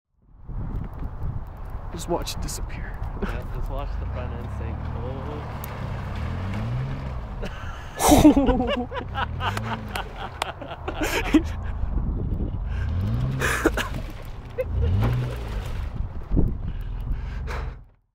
Edmonton 4x4 Jeep Liberty
Guy I ran into had never been to the place and decided to try a hole that he shouldn't have.